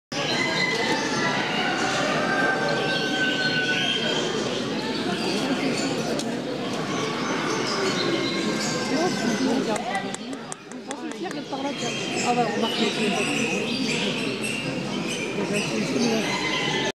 Capucin indigo bleu ecaillé
Exposition animaux de la ferme à neufchâtel en bray  week-end du  11 novembre  2013